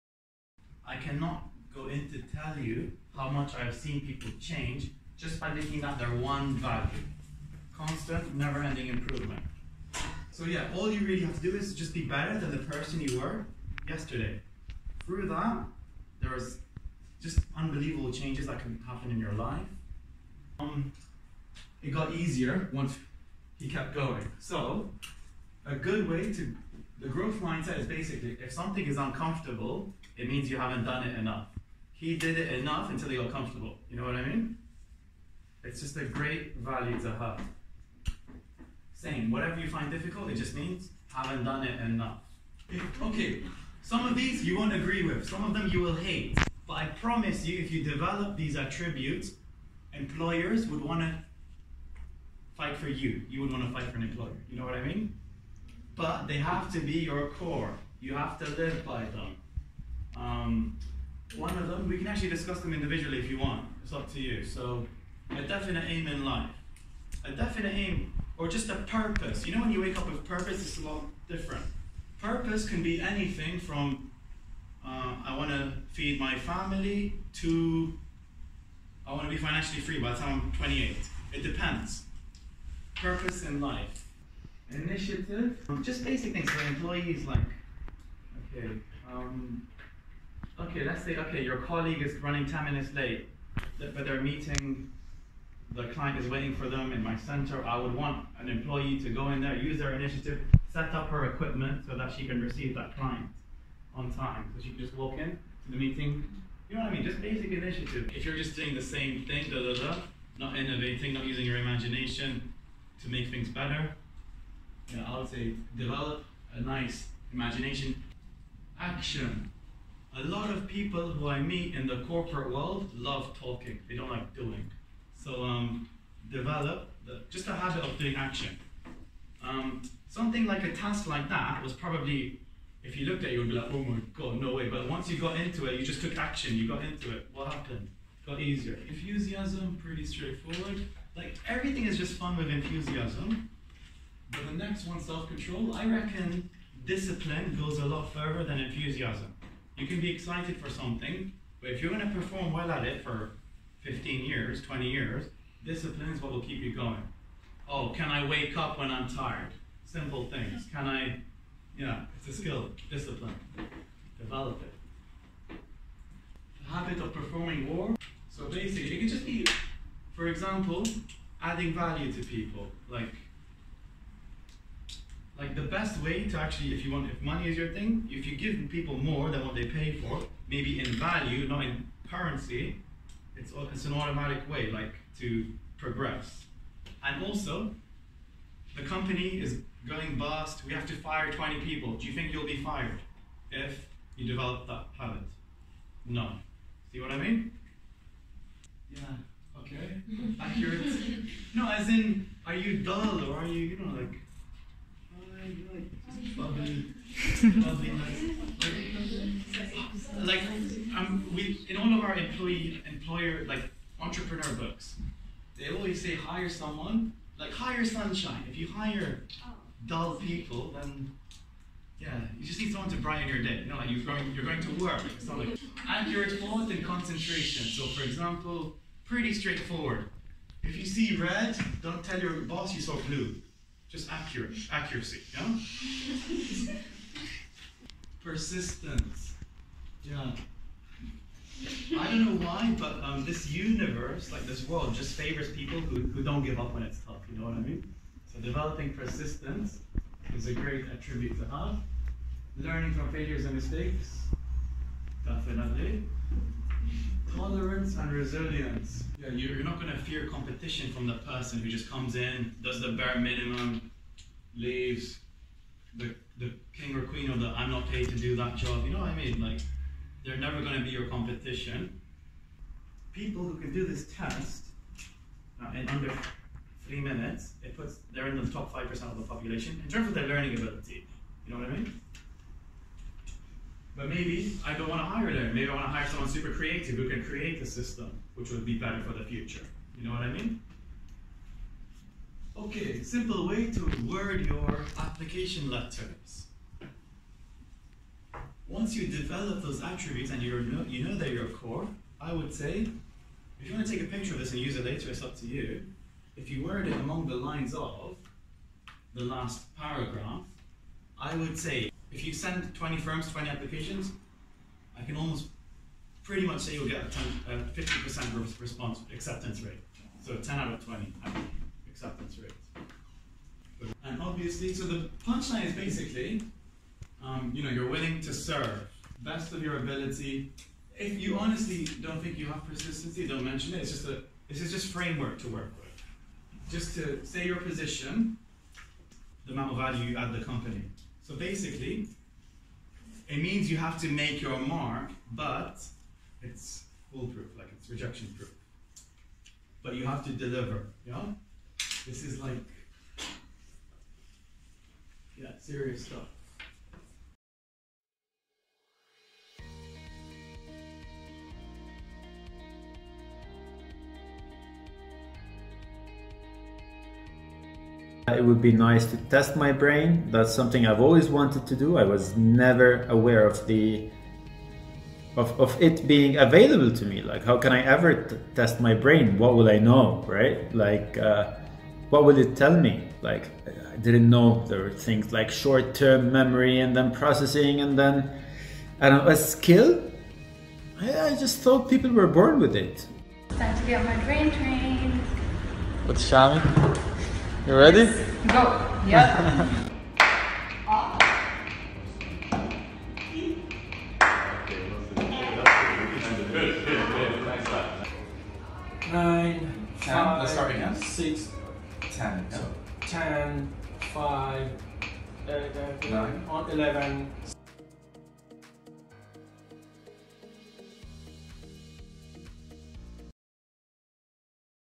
BEST Knowledge on Employability - Shammi Kapoor
Highlights from an old guest lecture on Employability for Careers Day at a local school. These can be applied to any tough climate. Our team is always happy to spend time with school / university students to bridge the gap between industry and academia.